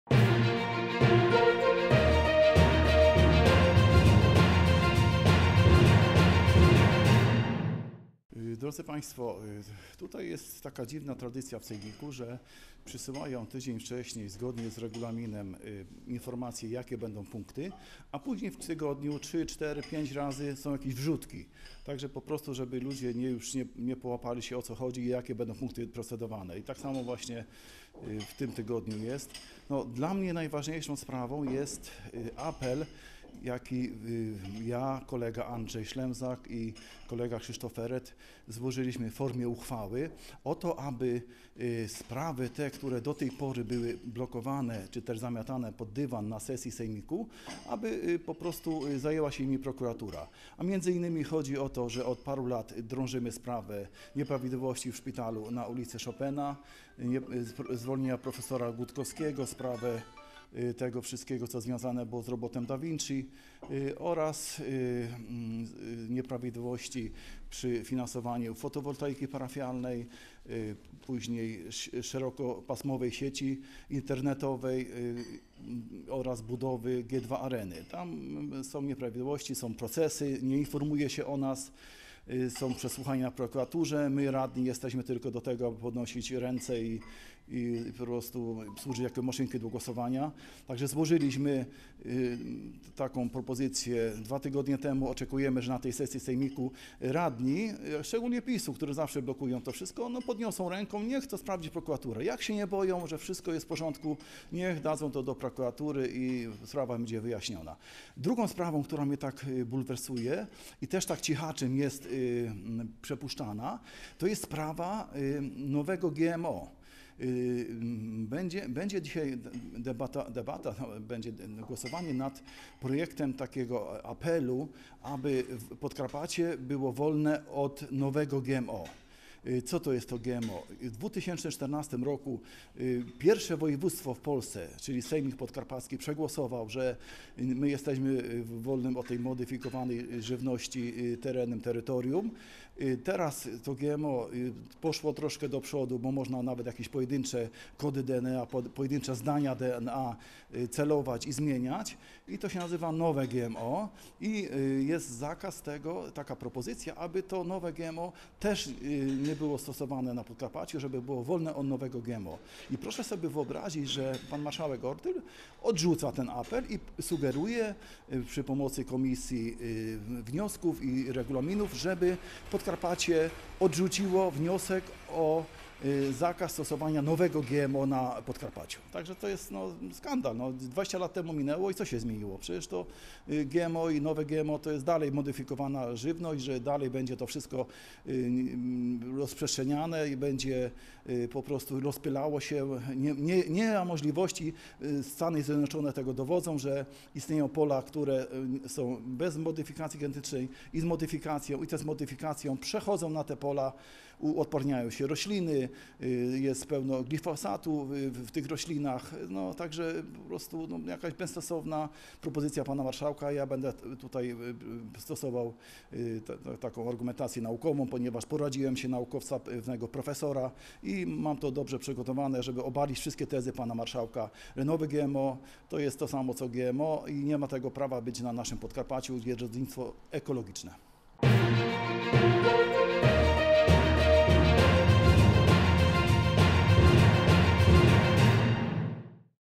JACEK KOTULA: "Apelujemy o zbadanie nieprawidłowości przez prokuraturę!"
Rzeszów/Podkarpacie. LXXI sesja Sejmiku Województwa Podkarpackiego VI kadencji.
Przed nasza kamerą Jacek Kotula Radny Sejmiku Województwa Podkarpackiego.